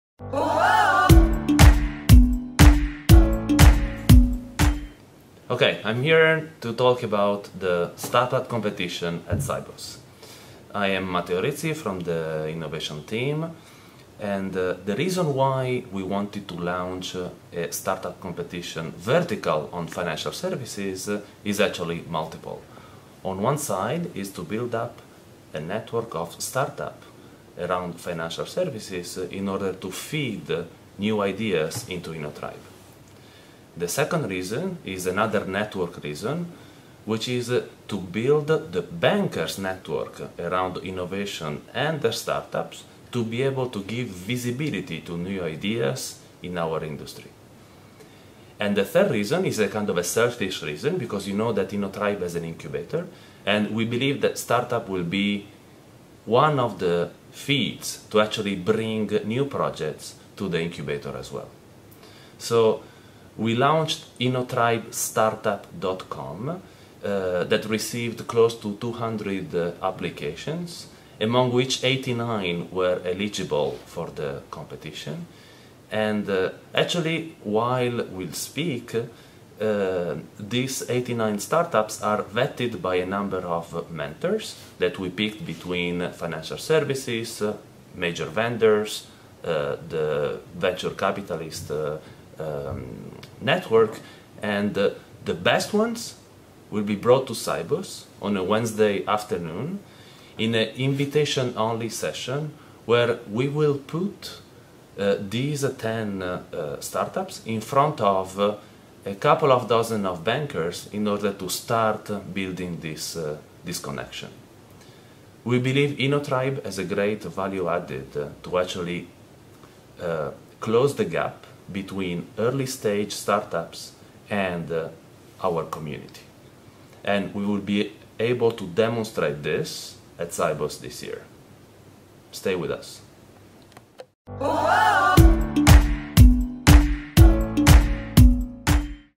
Innotribe@Sibos Toronto 2011 - Start-up competition lunch
Matteo introduces our start-up competition launch at Innotribe Sibos Toronto 2011!